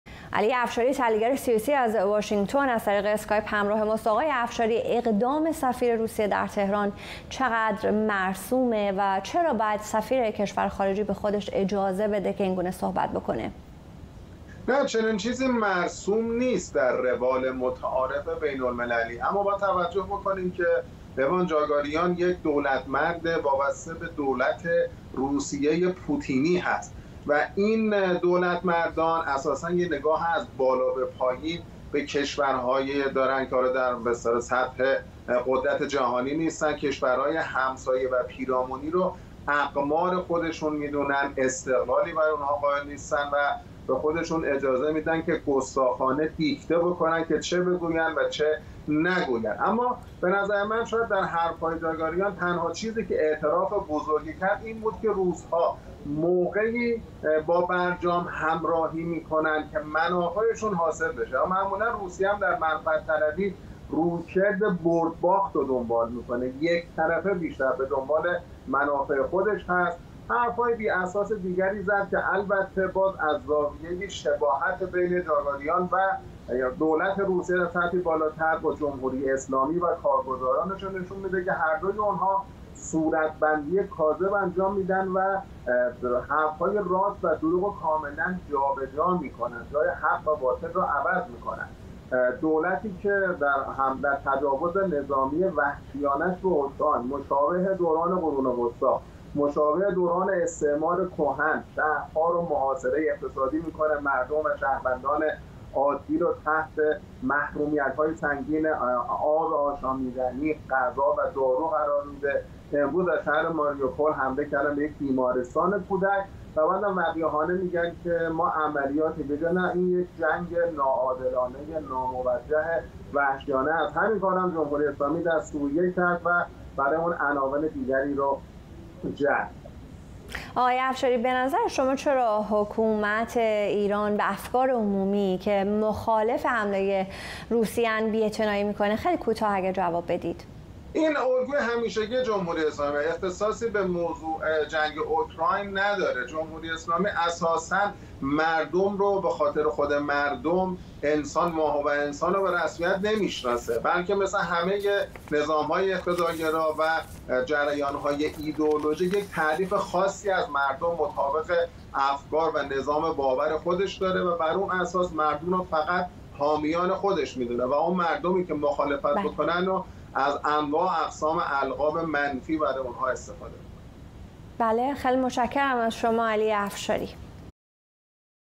چرا حکومت ایران به افکار عمومی که مخالف حمله نظامی روسیه به اوکراین است بی اعتنایی می‌کند؟
سفیر روسیه خطاب به رسانه‌های ایران: از عبارت حمله نظامی استفاده نکنید
اقدام سفیر روسیه چقدر در عرف بین‌المللی مرسوم است
گفت‌وگو با علی افشاری، تحلیلگر سیاسی